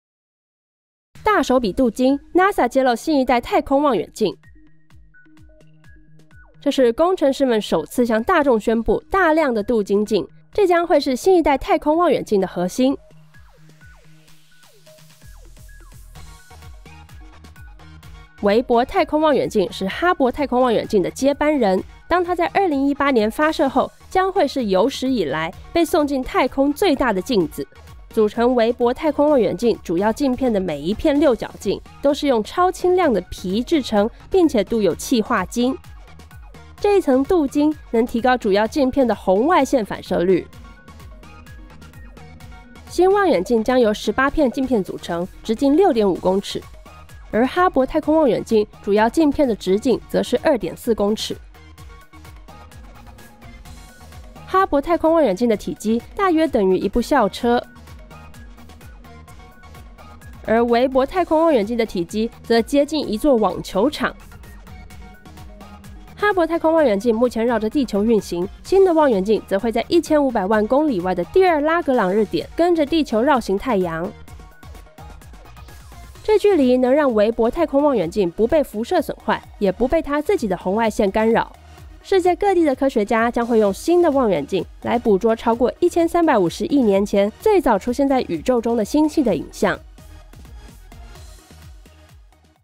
大手筆鍍金 NASA發表新一代太空望遠鏡
這是工程師們首次向大眾公佈，大量的鍍金鏡，這將會是新一代太空望遠鏡的核心。

韋伯太空望遠鏡（James Webb Telescope）是哈伯太空望遠鏡（Hubble Space Telescope）的接班人，當它在2018年發射後，將會是有史以來，被送進太空最大的鏡子。

組成韋伯太空望遠鏡主要鏡片的每一片六角鏡，都是用超輕量的鈹製成，並且鍍有氣化金。

這一層鍍金，能提高主要鏡片的紅外線反射率。

新望遠鏡將由18片鏡片組成，直徑6.5公尺。而哈伯太空望遠鏡，主要鏡片的直徑則是2.4公尺。

哈伯太空望遠鏡的體積，大約等於一部校車…

韋伯太空望遠鏡的體積，則接近一座網球場。

哈伯太空望遠鏡目前繞著地球運行，新的望遠鏡則會在1,500萬公里外的第二拉格朗日點（second Lagrange point），跟著地球繞行太陽。

這距離能讓韋伯太空望遠鏡，不被輻射損壞，也不被它自己的紅外線干擾。

世界各地的科學家，將會用新的望遠鏡，來捕捉超過1,350億年前，最早出現在宇宙中的星系的影像。